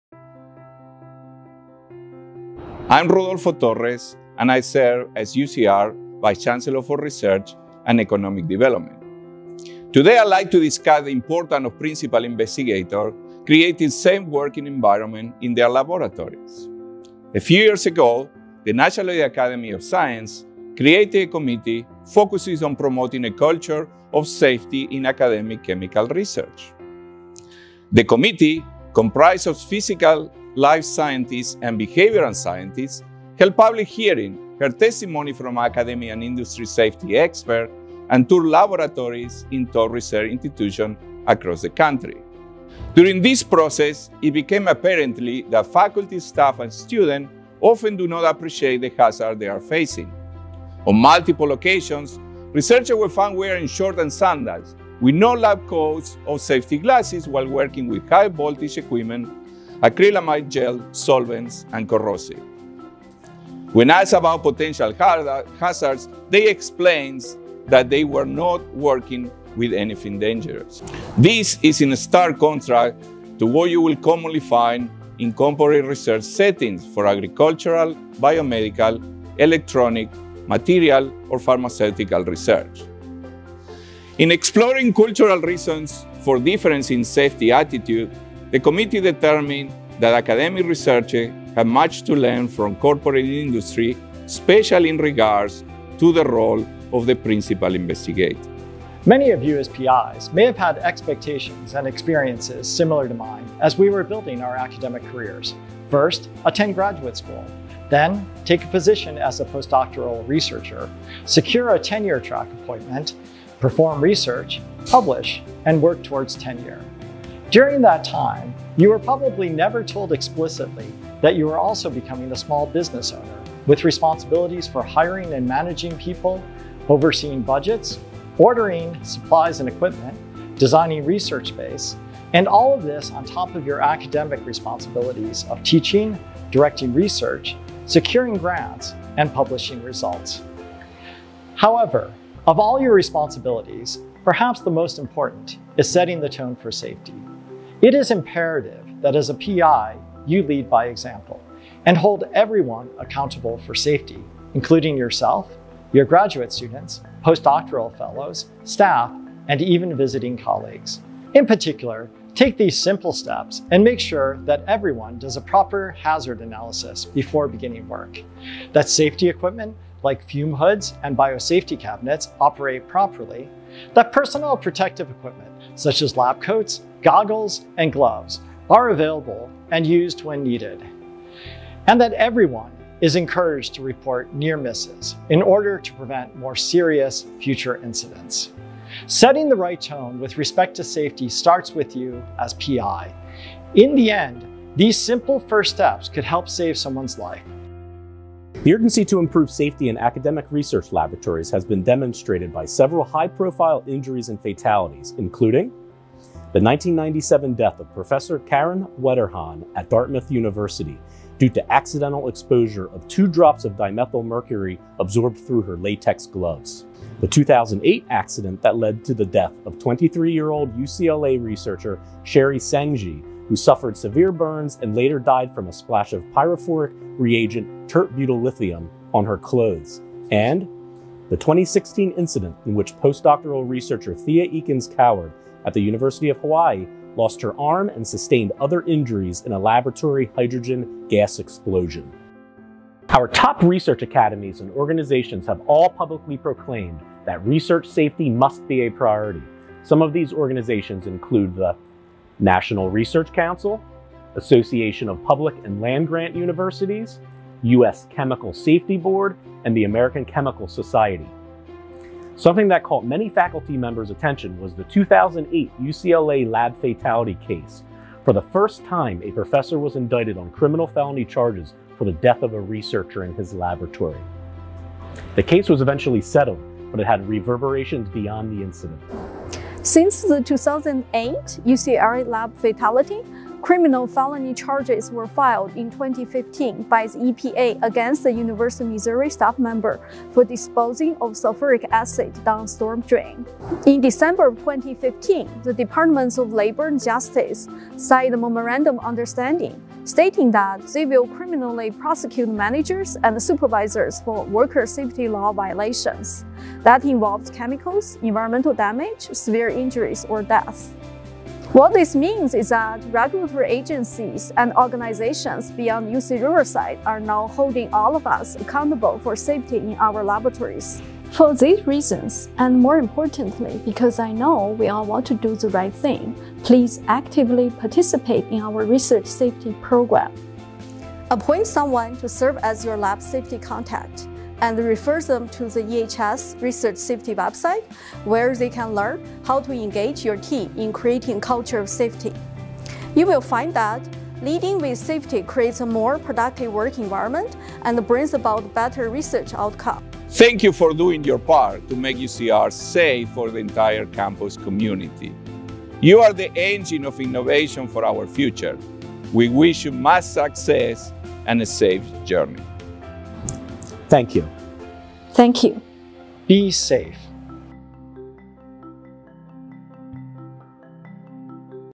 PI Responsibilities at UC Riverside
Leading with safety is key for a successful research program. Join us in building a culture of safety at UC Riverside, centered on Principal Investigator excellence in academic research.

Published: 07/11/2023